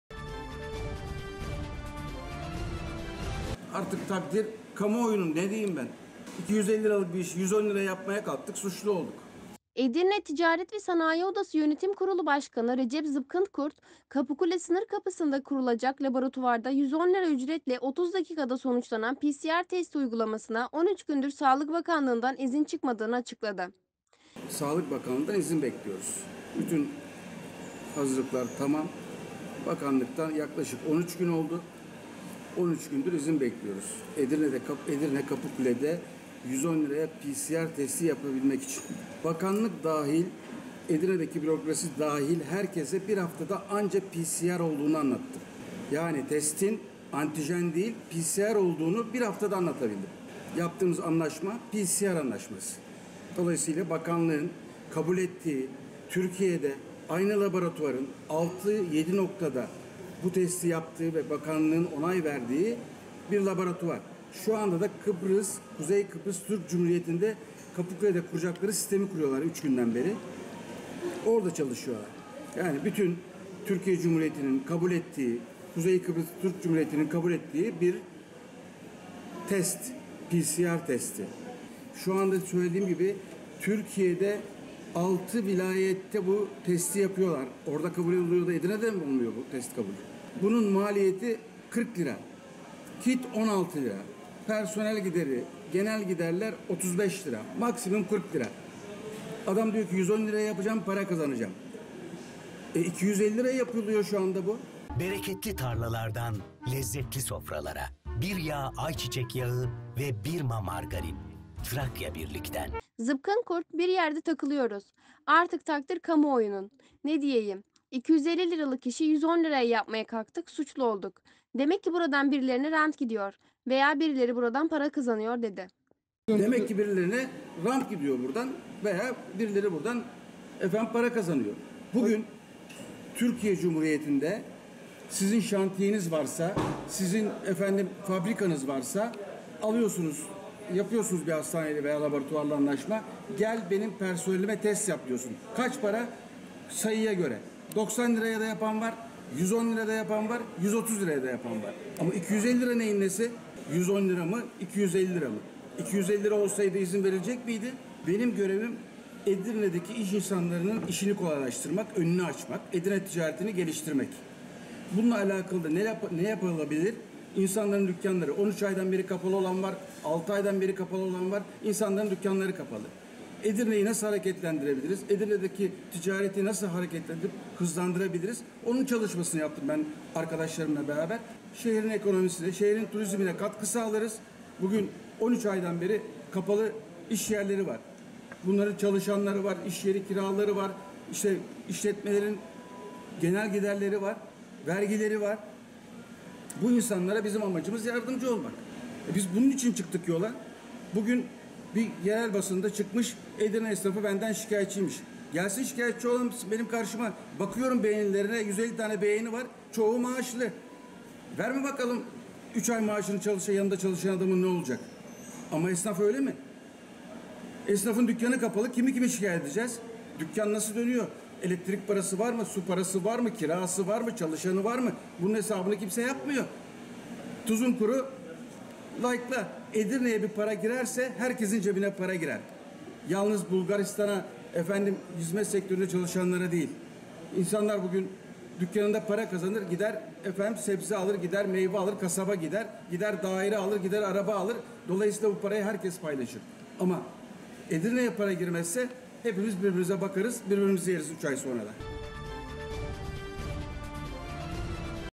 “250 liralık işi 110 liraya yapmaya kalktık, suçlu olduk”
Edirne Ticaret ve Sanayi Odası(ETSO) Yönetim Kurulu Başkanı Recep Zıpkınkurt, Kapıkule Sınır Kapısı'nda kurulacak laboratuvarda 110 lira ücretle 30 dakikada sonuçlanan PCR testi uygulamasına, 13 gündür Sağlık Bakanlığı’ndan, izin çıkmadığını açıkladı.  Zıpkınkurt,  “Bir yerde takılıyoruz. Artık takdir kamuoyunun, ne diyeyim. 250 liralık işi 110 liraya yapmaya kalktık, suçlu olduk. Demek ki buradan birilerine rant gidiyor veya birileri buradan para kazanıyor” dedi.